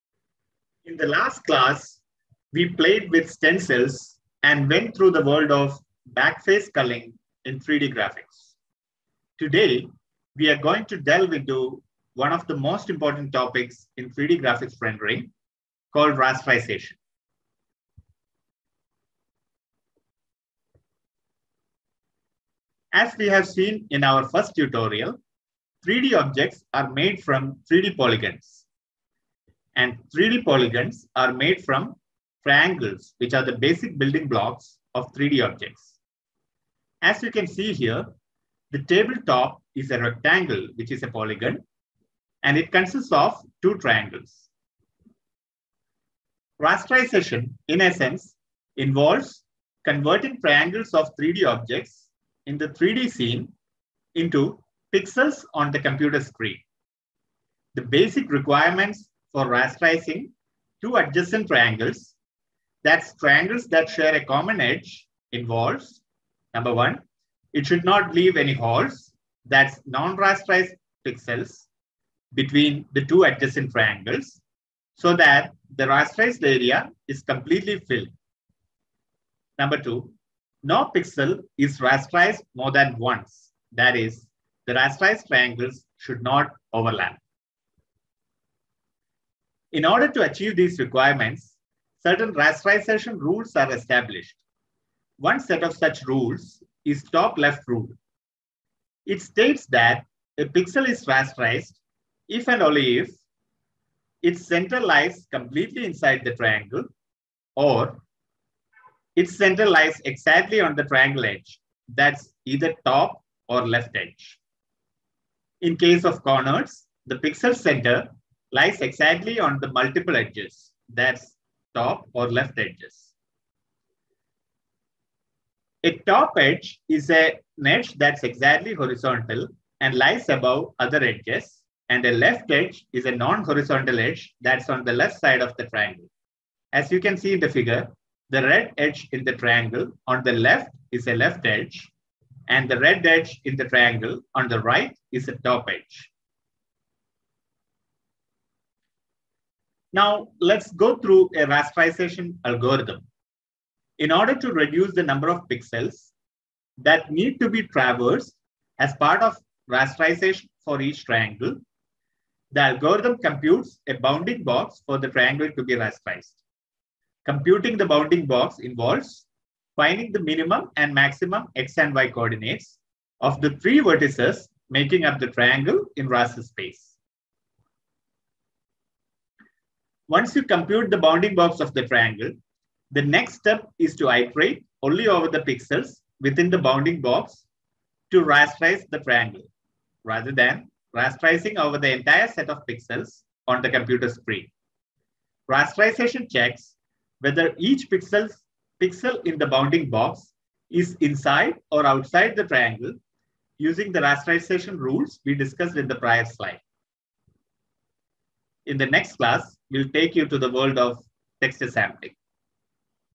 3D Graphics - Rasterization
This session takes you through one of the most important topics in 3D graphics, Rasterization.